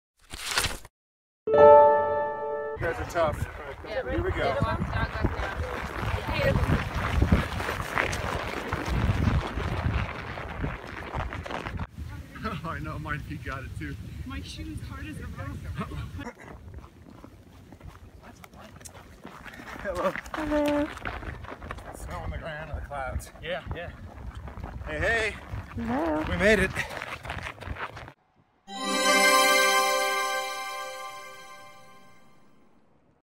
Cold run in Colorado Springs on a snowy day
Gazette's Kaitlin Durbin gets up at oh-dark-hundred to watch the Sunrise Striders make a frigid run at Bear Creek Park in Colorado Springs.